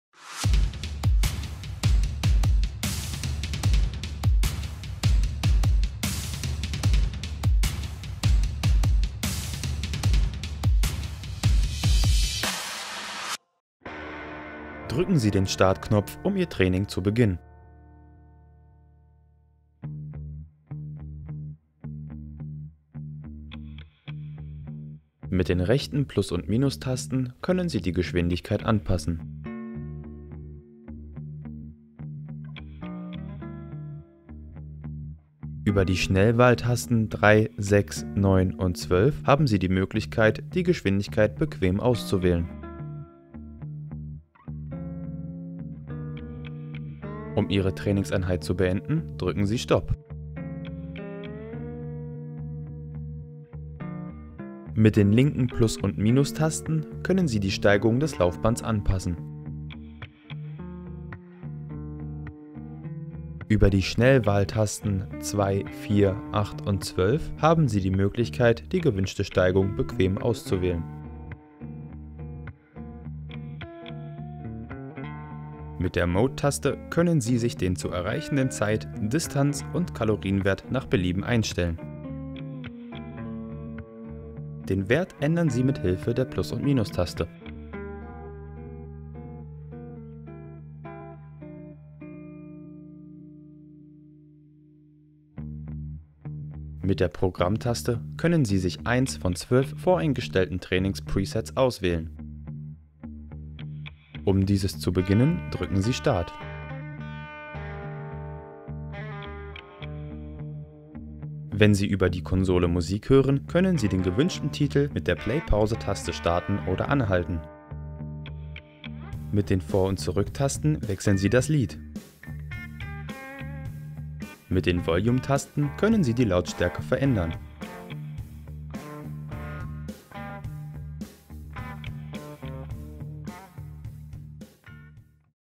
Sportstech F31 - Konsolenbedienung
In diesem Video zeigen wir Ihnen, wie Sie die Konsole Ihres F31 bedienen.